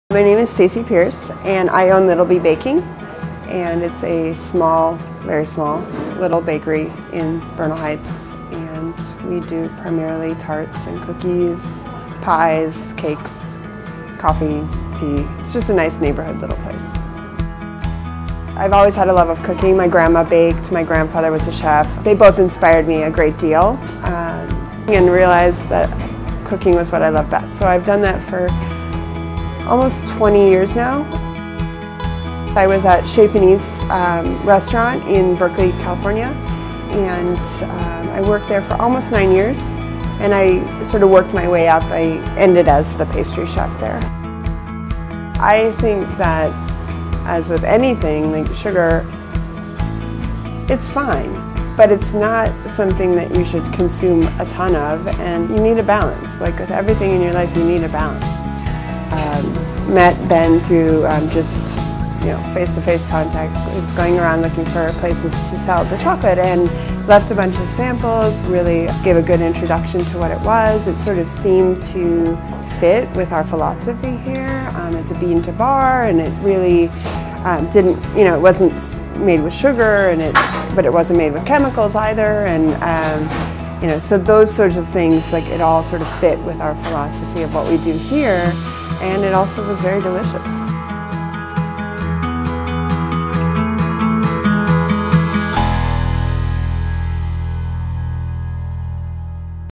The Good Chocolate - Stacie Pierce
Stacie Pierce is the pastry chef and founder of Little Bee Baking in San Francisco.